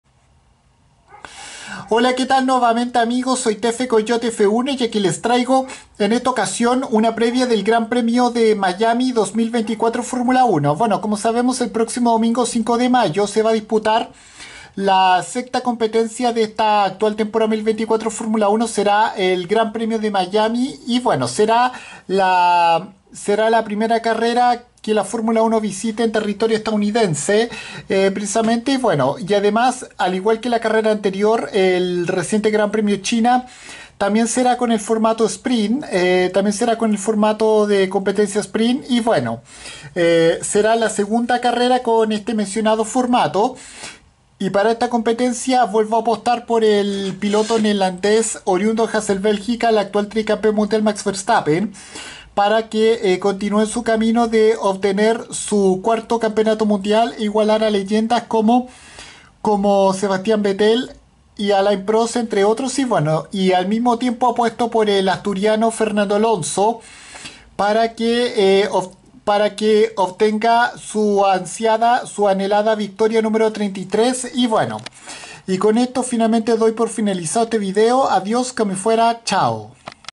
Previa GP de Miami 2024 de F1
Una breve previa de lo que será el Gran Premio de Miami de la Temporada 2024 de la Fórmula 1 que se correrá alrededor del Hard Rock Stadium

Para esta competencia volveré a apostar por el neerlandés oriundo de Hasselt Bélgica actual tricampeón mundial Max Emilian Verstappen para que siga su camino de igualar en campeonatos mundiales a leyendas como Sebastian Vettel o Alain Prost